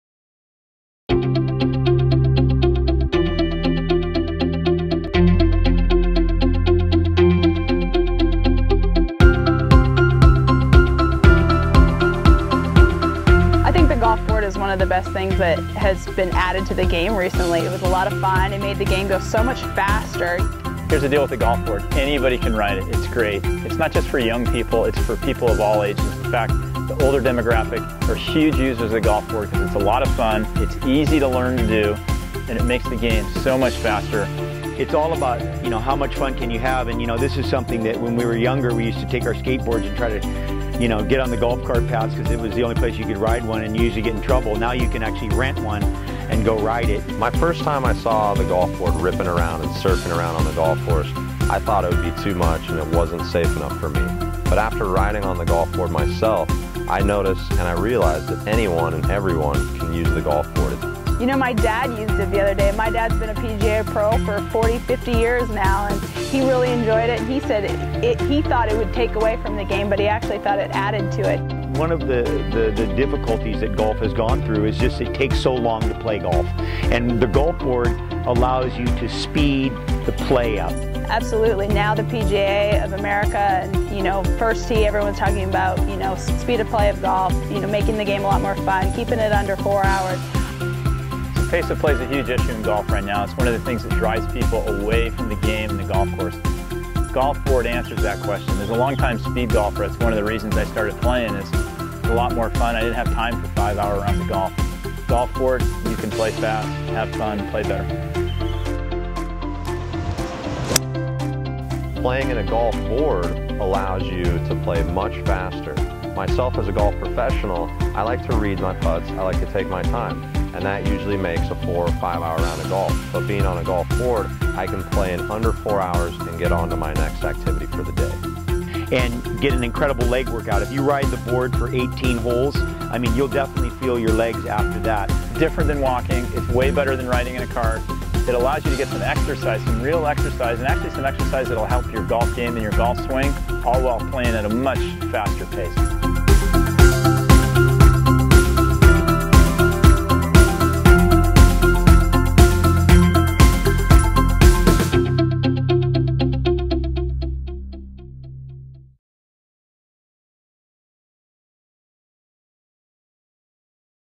Meet The Golf Board - A Way To "Surf" Around The Golf Course
The Golf Board is a 4 wheel transporter for one person. It can travel up to 14 mph and will cost you a few thousand dollars.  Golfers say it's more convenient than using a golf cart.  What do you think?